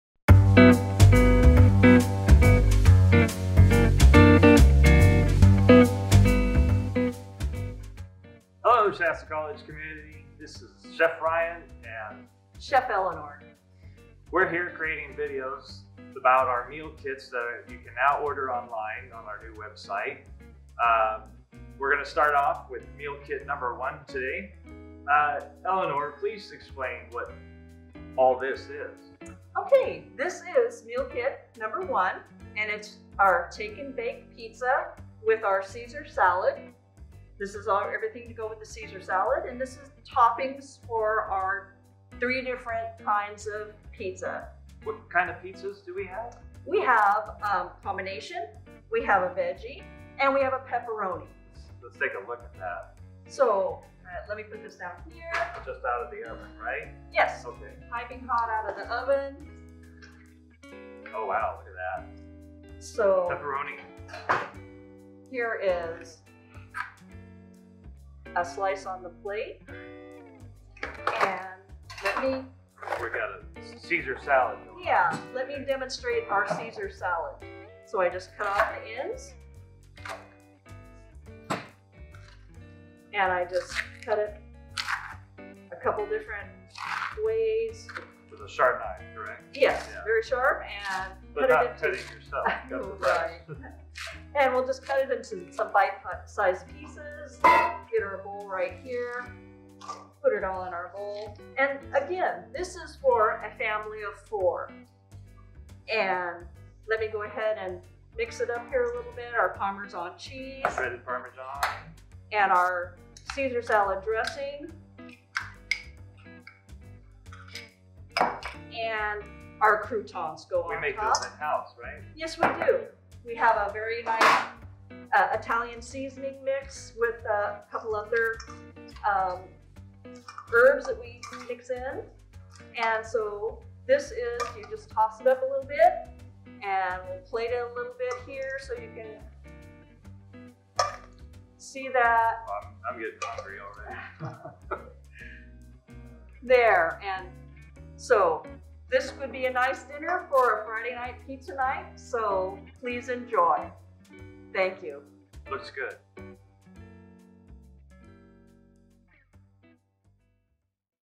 Shasta College Food Services Meal Kits: Take n' Bake Pizza
Watch our Food Services Chefs walk you through our awesome Take n' Bake Pizza Meal Kit. Our Meal Kits feed a family of four.